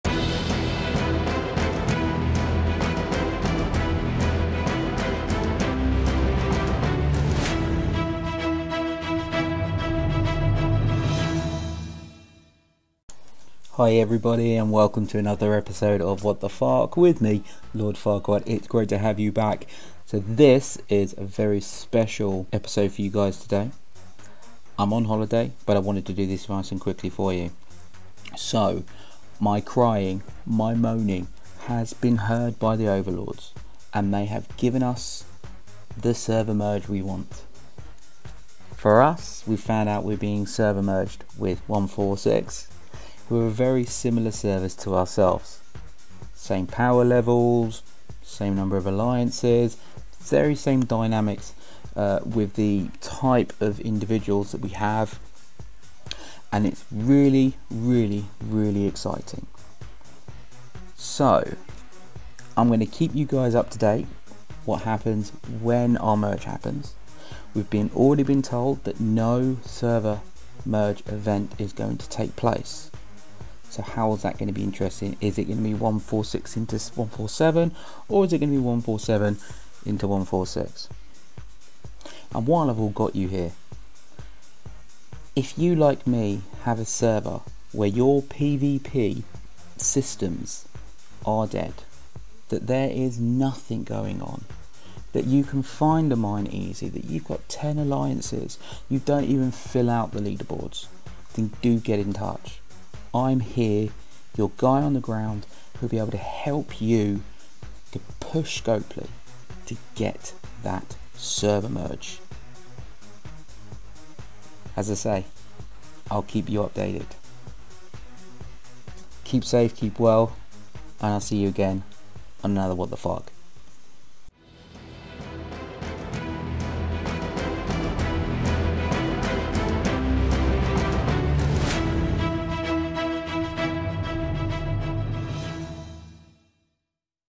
STFC - Holy **** Did you say Server Merge
The STFC overloads at Scopely have heard the crying from me and given in to my demands.
Yes it is finally happening, 147 are getting our merge !